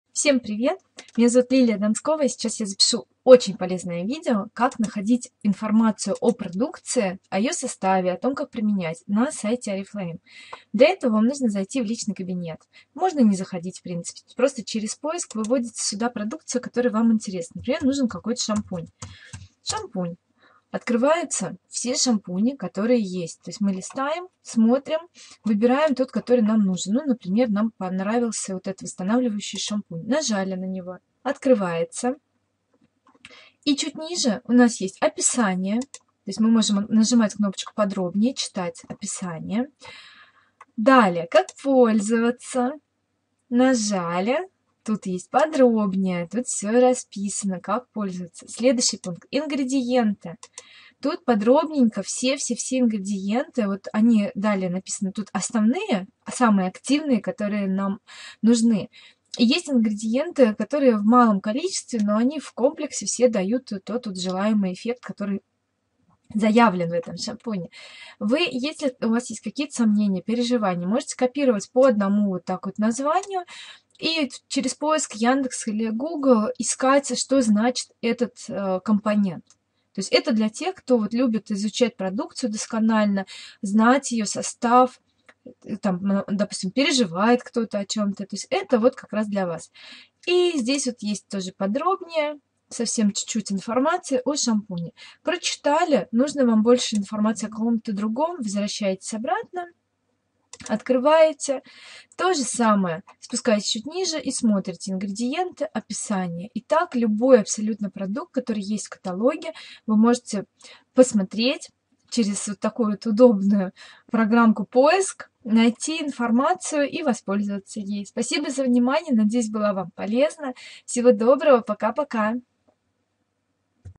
КАК ИСКАТЬ ИНФОРМАЦИЮ о продукции на сайте Oriflame
Объясняю как искать информацию о продукции на сайте Oriflame.
Надеюсь, будет полезно всем))